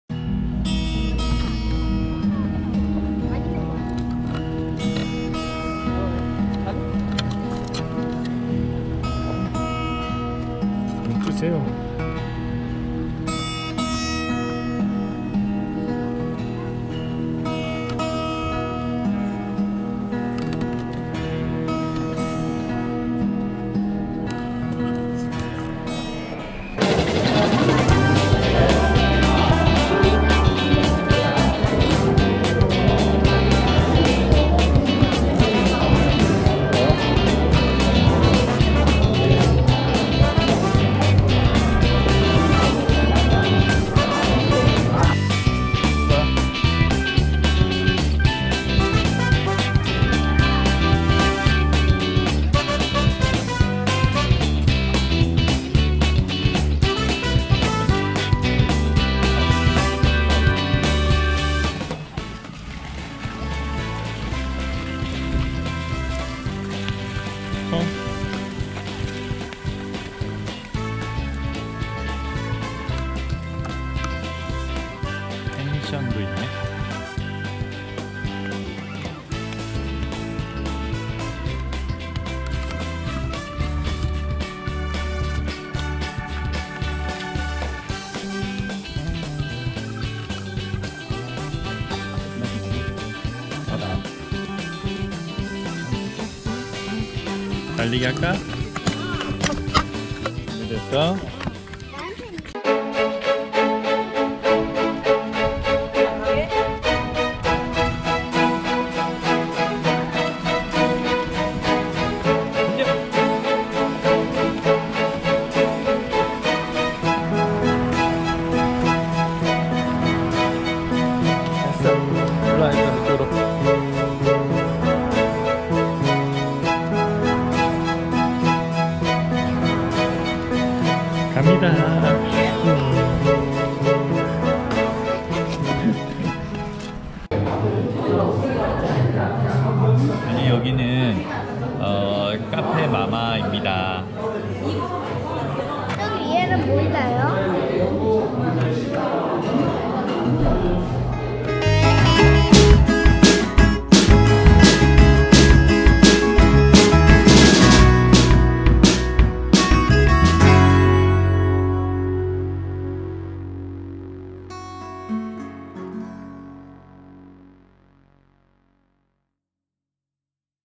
카페 마마, 가족나들이, cafemama
즐거운가족나들이
정원이 있는 카페들은 참 이뻐요

안녕하세요 착한PD입니다.
함께 성장해요~
착한피디 오픈 카카오 채팅방 :

- 후원계좌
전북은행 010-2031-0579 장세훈